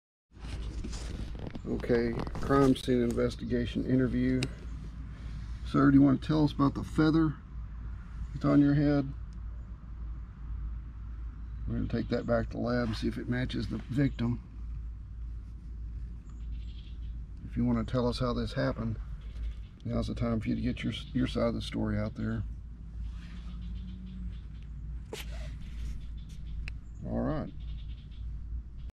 CRIME SCENE CAT, Evidence collection and interview.
Claude waved his right to remain silent, then remained silent.  This time the charges may stick if the evidence matches the missing dove.